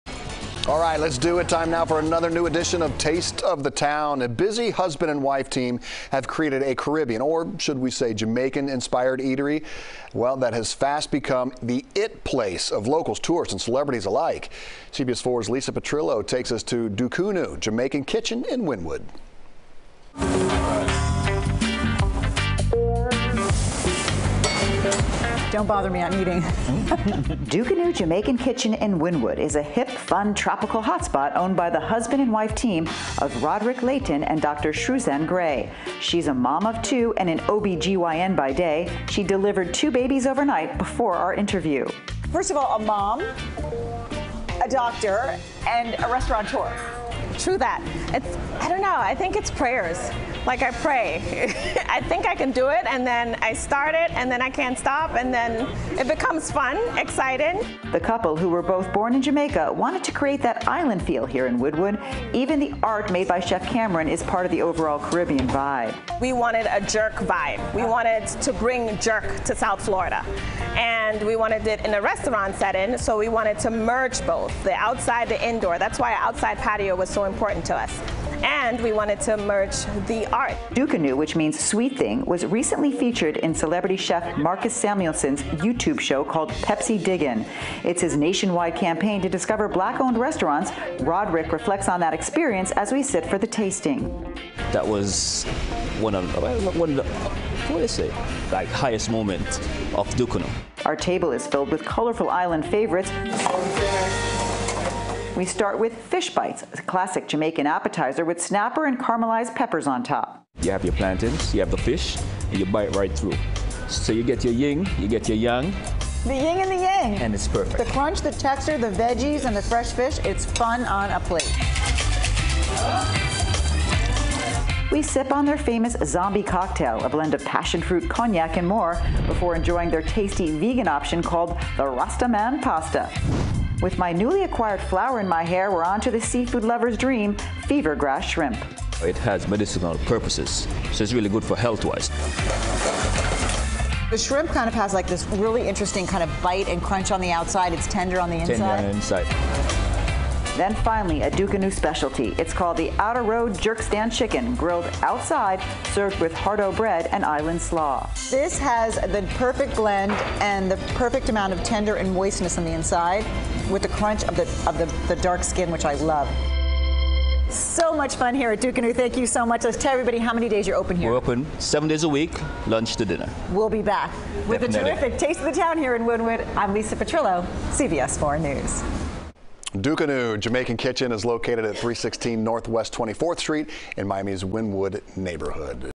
Taste Of The Town: Dukanoo Jamaican Kitchen Serves Up Flavors And Fun
Lisa Petrillo reports Dukunoo Jamaican Kitchen in Wynwood is a hip, fun tropical hotspot.